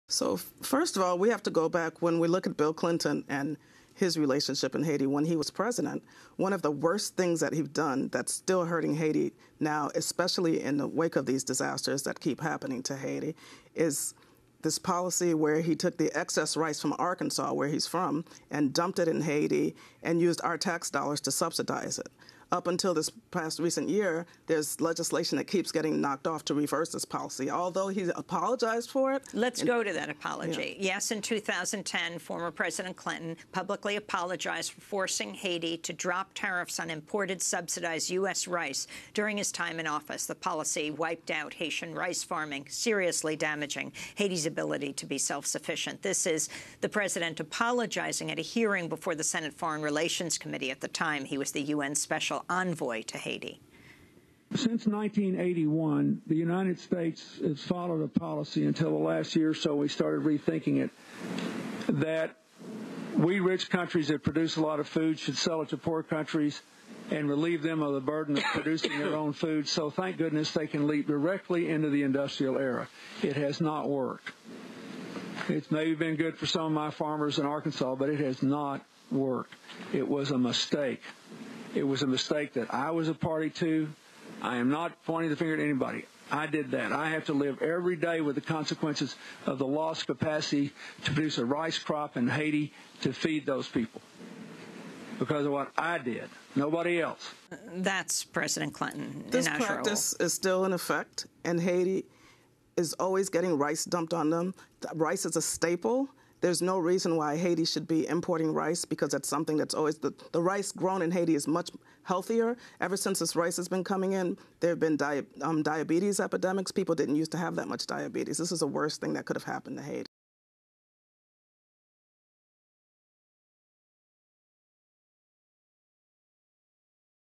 Las políticas comerciales de Bill Clinton tienen a Haití enfrentando hambruna tras el huracán Mathew
En el año 2010, el ex presidente Bill Clinton pidió perdón públicamente por haber obligado a Haití a reducir los aranceles al arroz subsidiado importado de EE.UU. durante su gobierno. La medida aniquiló el cultivo de arroz y dañó seriamente la capacidad de Haití de ser autosuficiente. “Quizás fue bueno para algunos de mis productores agropecuarios de Arkansas, pero no funcionó. Fue un error”, dijo Clinton en el año 2010. “Debo convivir con las consecuencias de lo que hice, que Haití haya perdido la capacidad de producir un cultivo de arroz para alimentar a esa gente”. Seis años después del pedido de disculpas de Clinton, Haití enfrenta una nueva crisis alimentaria tras el paso del huracán Matthew.